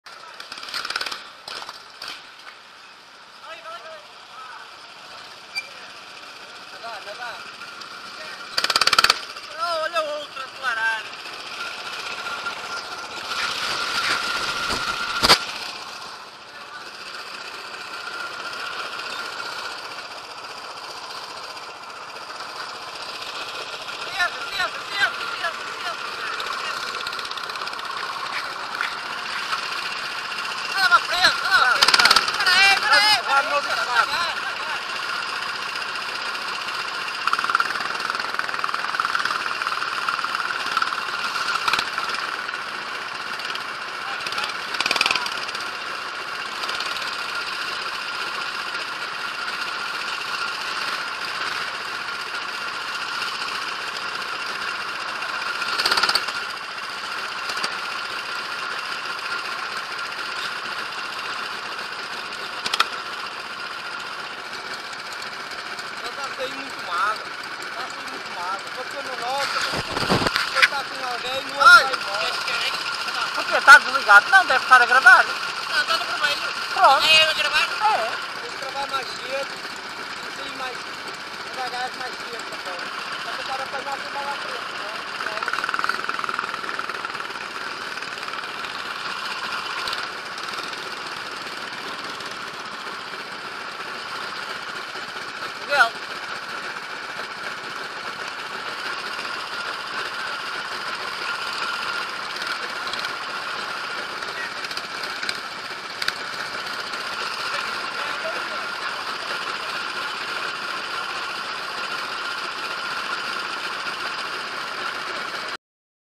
Cavaleiros do Asfalto nas 24 Horas de Karting de Braga - 2011 - Filme 3
Preparadinho para arrancar para o penúltimo turno!